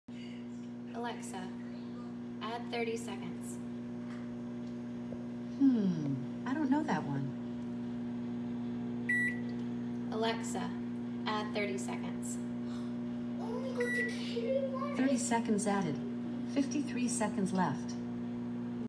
trying to add 30 seconds
"Alexa, add 30 seconds" didn't work while microwave was running for some reason.